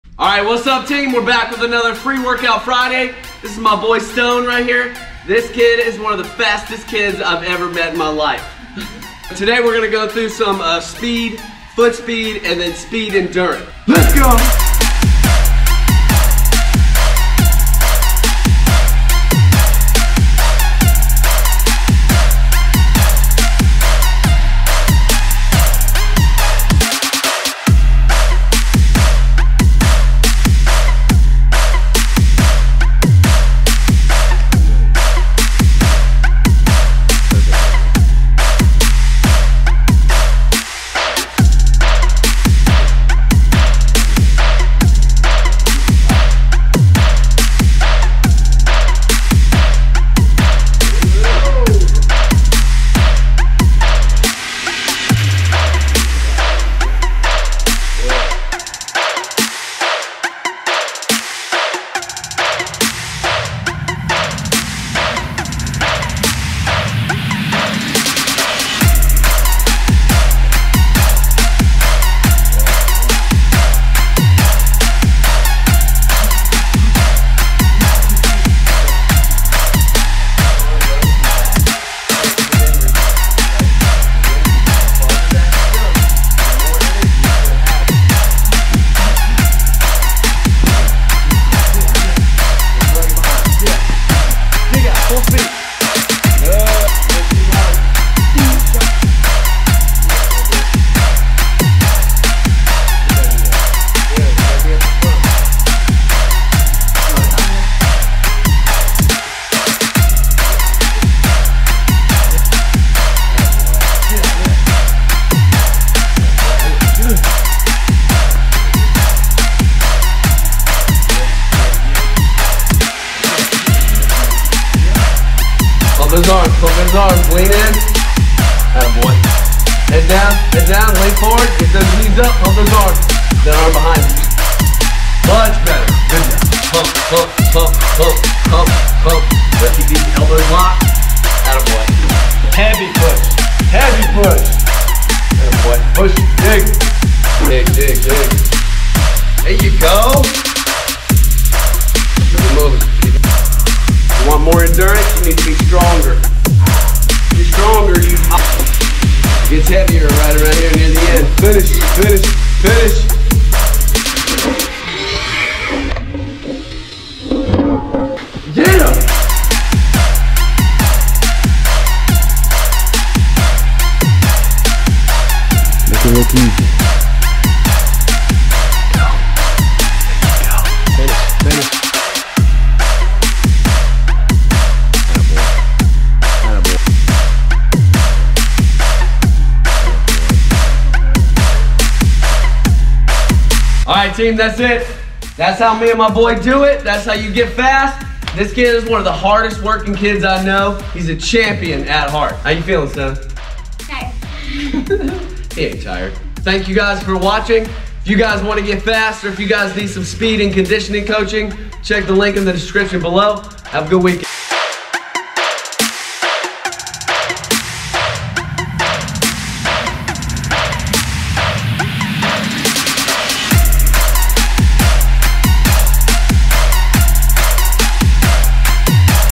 free workout fridays #5 - Foot Speed & Speed Conditioning for Young STUD
This is one of the fastest kids ive ever known and his is only getting FASTER!

Back again with another free workout friday!

My boy Stone is a CHAMPION and he trains like one too!

Today Stone will be working on a few things that will improve his overall speed, speed endurance and strength as an athlete! Including...
- foot speed
-sprinting technique
-speed endurance conditioning
-and muscle building
-explosiveness!

Youth training for sports and youth athletes are some of my FAVORITE training and I absolutely LOVE IT!!!